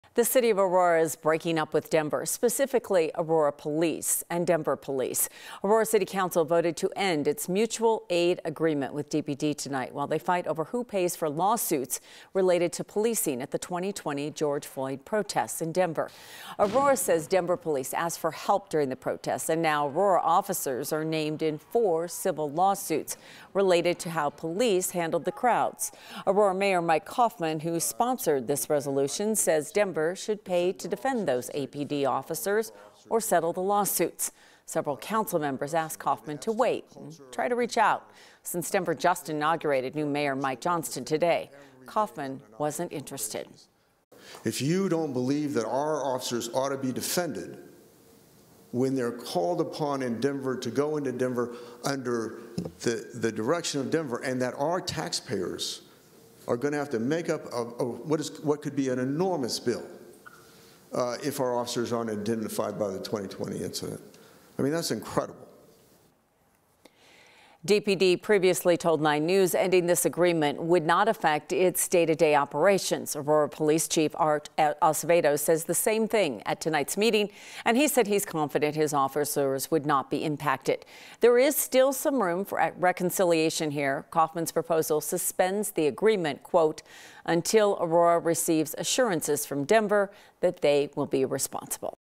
Aurora votes to suspend mutual aid deal with Denver Police
The city of Aurora voted Monday night to break up with Denver, ending its mutual aid agreement with Denver's police department.

Aurora's city council voted to suspend its agreement with DPD Monday as they continue to argue over which municipality should pay for lawsuits related to policing at the 2020 protests in Denver.

9NEWS (KUSA) is located in Denver, Colorado.